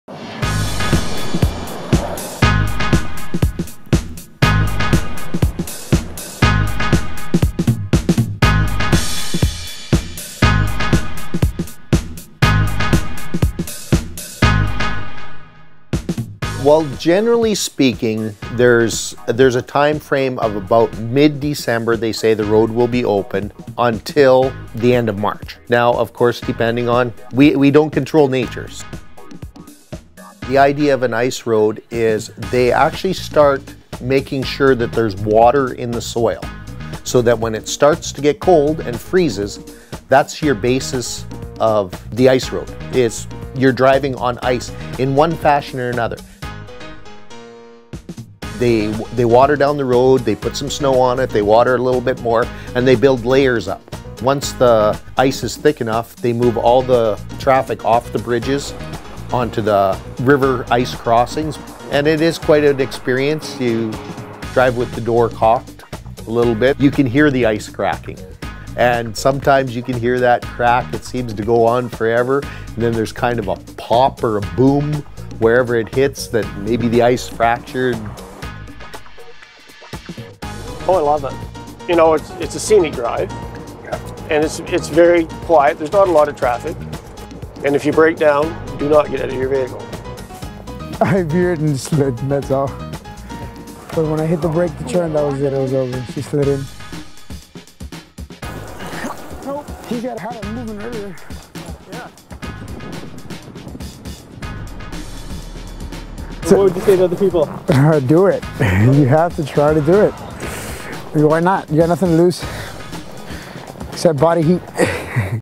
Driving the winter ice road to Fort Chipewyan
Take a ride up the winter ice road from Fort McMurray to Fort Chipewyan in Northern Alberta. The 200-km temporary road typically opens mid-December and closes mid-March depending on the weather. During that short window of time all the construction materials, heating oil, gasoline and diesel fuel for the year is shipped up to the isolated community of Fort Chipewyan which can only be accessed by air or river barge in the summer.  Video by Ryan Jackson, edmontonjournal.com.